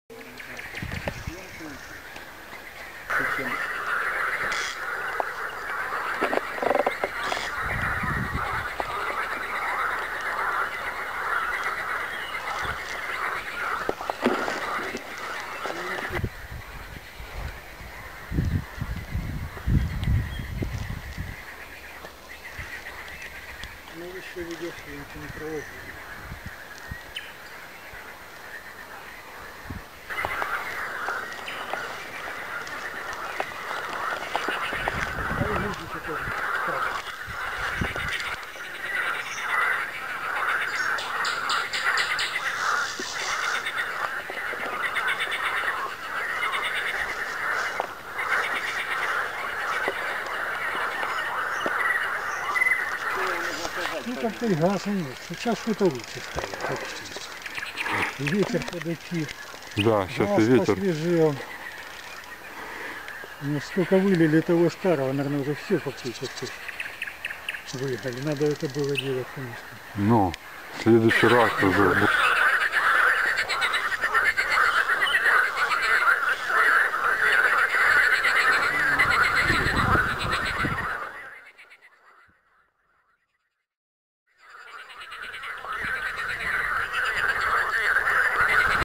Eco-balloon in Ukraine, spring 2013
Location: National park at Eastern Ukraine. We test-fly a small balloon with camera, designed for ecological mapping. Soundtrack is authentic, with singing frogs and nightingale.